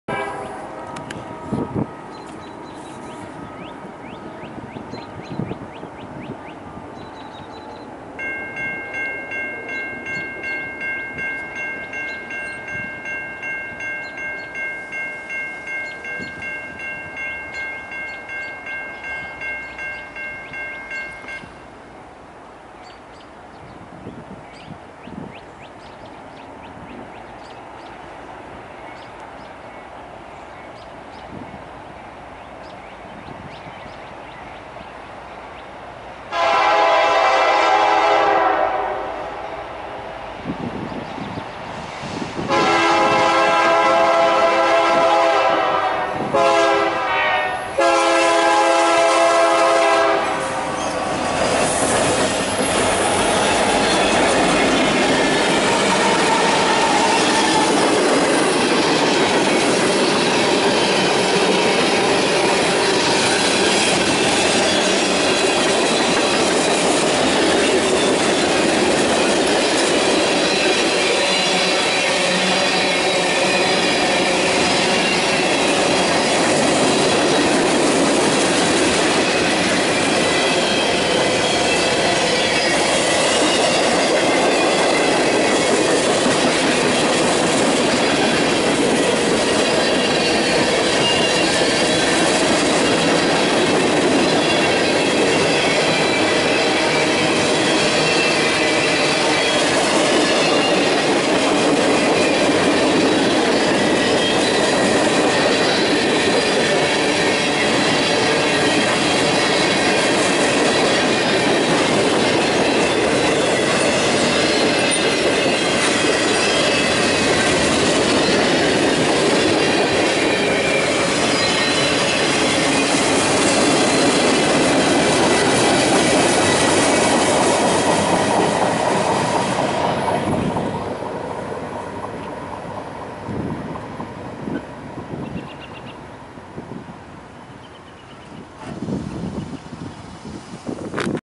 NS 7254 West Sandusky, OH
WB Manifest, Mills St., Sandusky, OH
NS 7254 = SD9043MAC - UP WWD - Older K3HA
NS 7290 = SD9043MAC - UP WWD
UP 4908 = SD70M - Flags
UP 5595 = AC4400CW-CTE - Flags